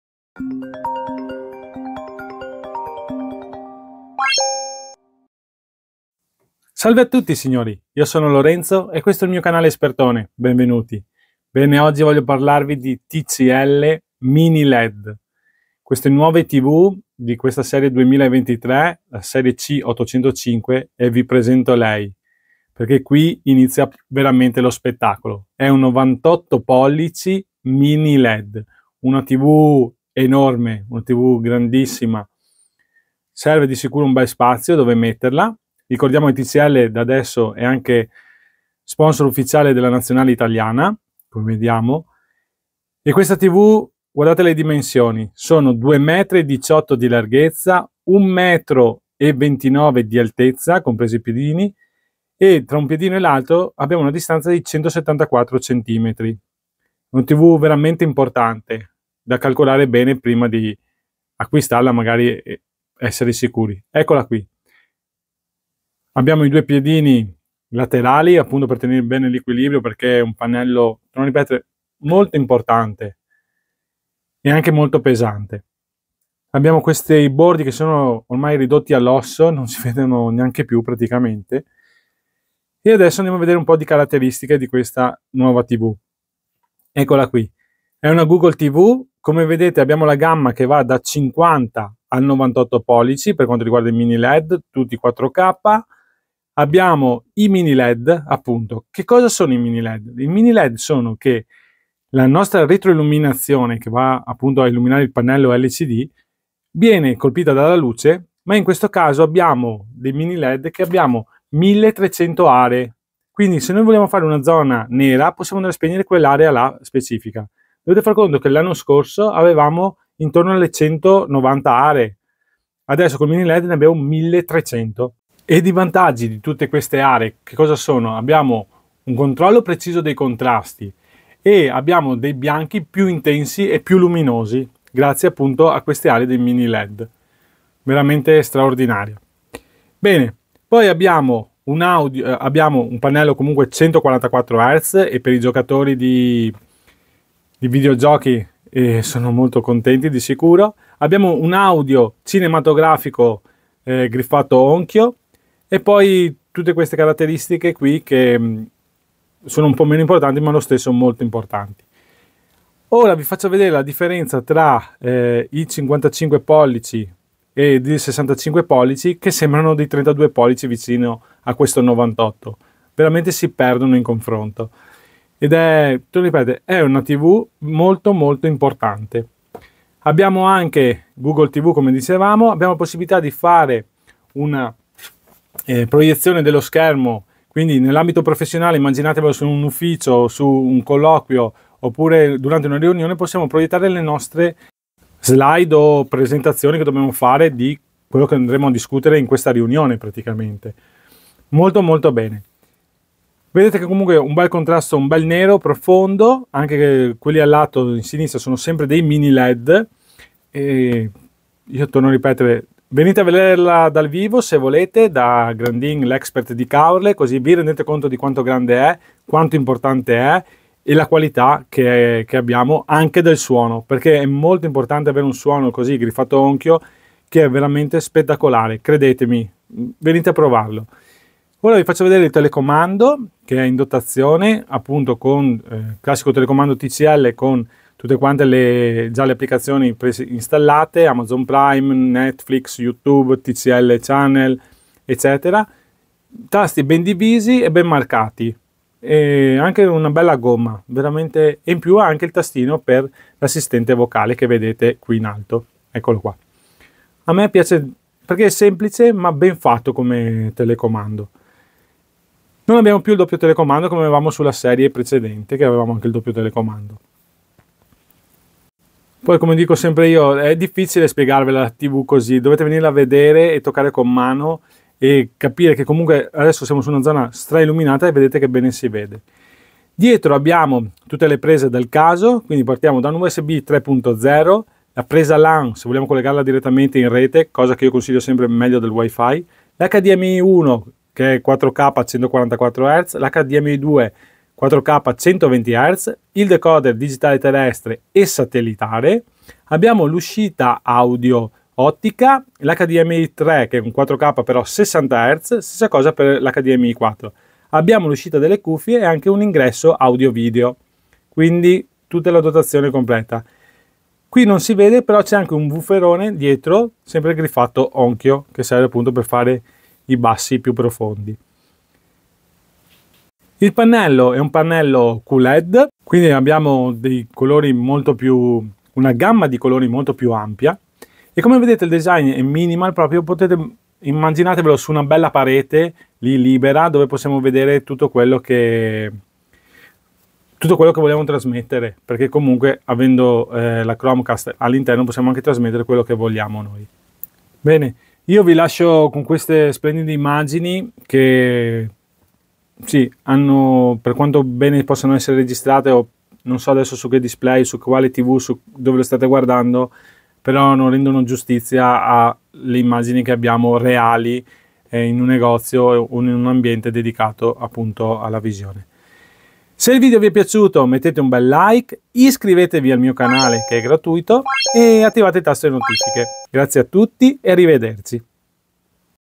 Novità 2023 TV TCL 98C805, miniled, 144hz, google tv, uno schermo enorme.
Novità 2023 , la nuova TV mini led ben 98 pollici uno schermo enorme , 144 hz per gli amantibdel gaming e non , google tv per condividere lo schermo e scaricare tutte le applicazioni.
Se volete acquistarlo

Ringrazio
Expert di Caorle
Strada Sansonessa 39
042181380